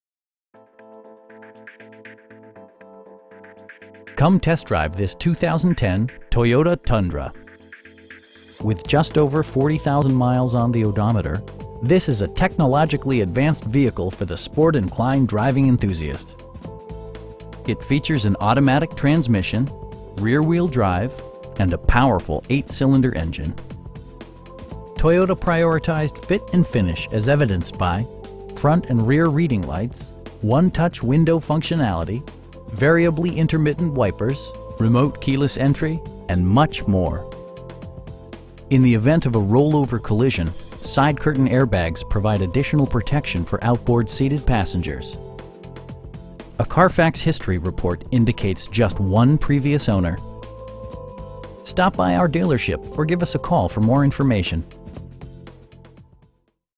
2010 Toyota Tundra Grade 4.6L V8 in Nashville, TN 37203
Beaman Toyota
1525 Broadway (Downtown Nashville)

Come test drive this 2010 Toyota Tundra. With just over 40,000 miles on the odometer, this is a technologically advanced vehicle for the sport-inclined driving enthusiast! It features an automatic transmission, rear-wheel drive, and a powerful 8 cylinder engine. Toyota prioritized fit and finish as evidenced by: front and rear reading lights, 1-touch window functionality, variably intermittent wipers, power door mirrors, remote keyless entry, an overhead console, and much more. In the event of a rollover collision, side curtain airbags provide additional protection for outboard seated passengers. A Carfax history report indicates just one previous owner. We pride ourselves on providing excellent customer service. Stop by our dealership or give us a call for more information.